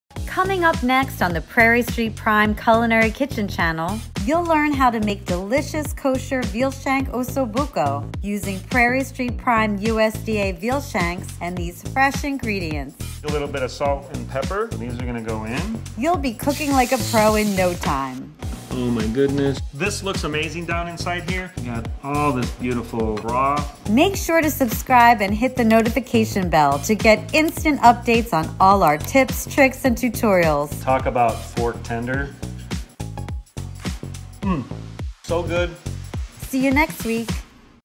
Trailer for Kosher Veal Osso Bucco Recipe
Kosher Veal Shank Osso Bucco
by Chef Randy

The Prairie Street Kosher Veal Osso Bucco makes for a rich and luxurious meal any night of the week.  The bone marrow, connective tissue, and marbling on this cut of meat marry the earthy flavors of Chef Randy’s Osso Bucco when cooked down.   With help from the electric pressure cooker, you can take a typical 4 to 5 hour braise down to just under 90 minutes for a hearty dish your entire family will love.